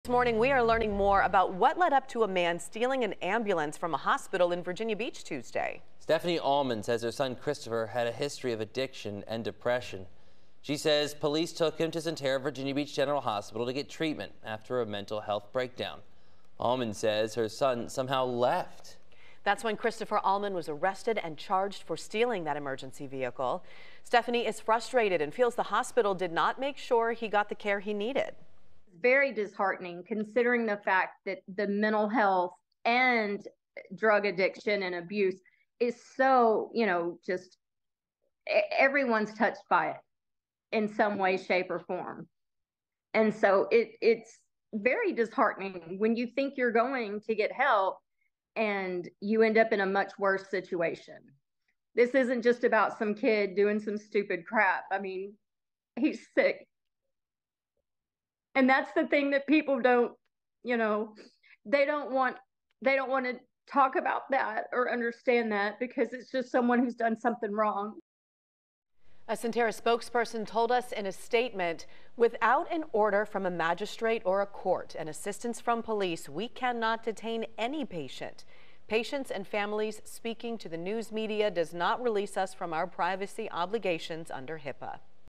The mom of the suspect accused of stealing an ambulance, speaks out
The mom of the suspect accused of stealing an ambulance, speaks out. She said he son was trying to get the help he needed.